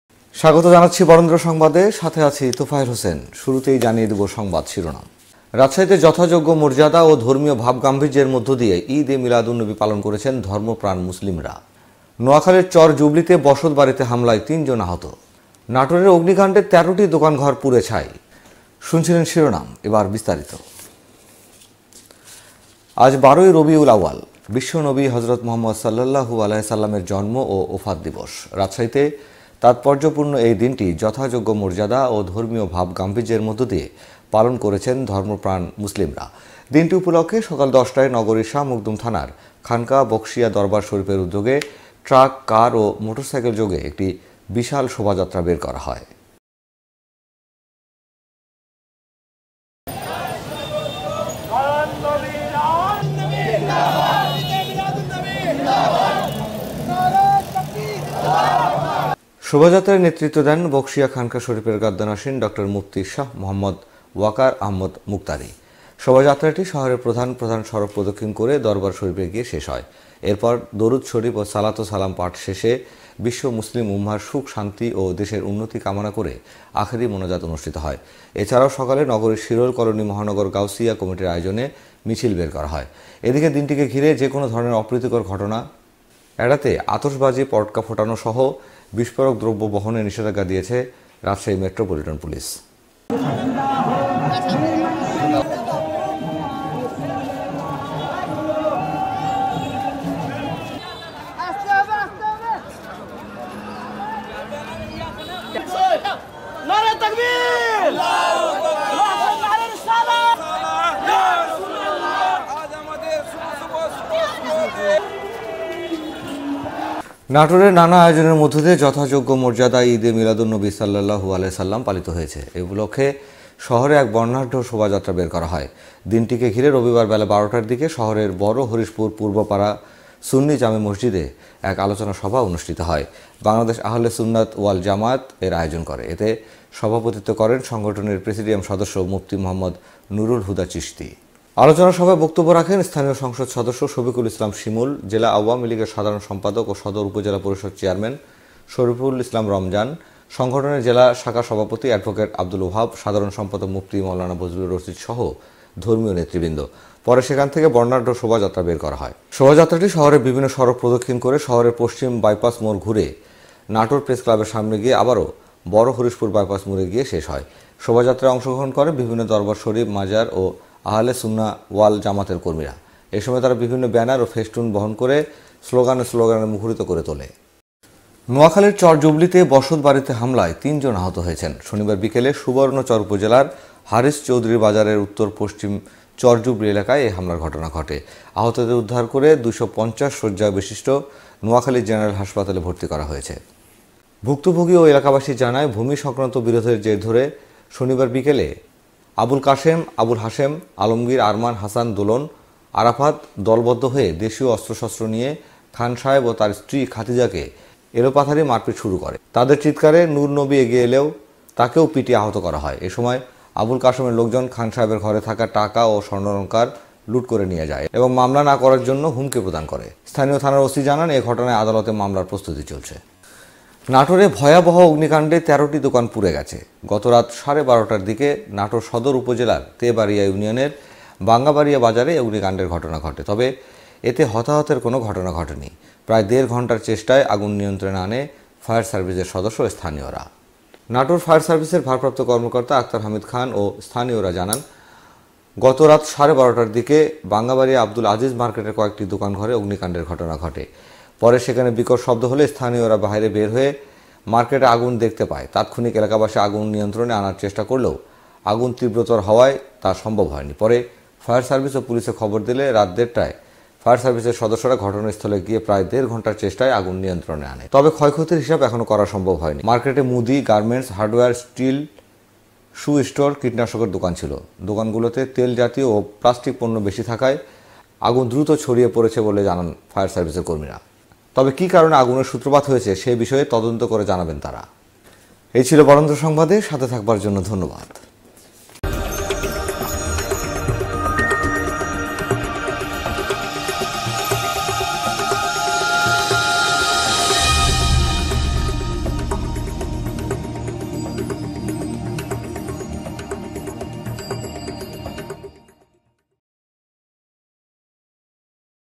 যথাযোগ্য মর্যাদা ও ধর্মীয় ভাবগাম্ভীর্যের মধ্যদিয়ে ঈদ এ মিলাদুন্নবী পালন করেছন ধর্মপ্রাণ মুসলিমরা
রাজশাহীতে যথাযোগ্য মর্যাদা ও ধর্মীয় ভাবগাম্ভীর্যের মধ্যদিয়ে ঈদ এ মিলাদুন্নবী পালন করেছন ধর্মপ্রাণ মুসলিমরা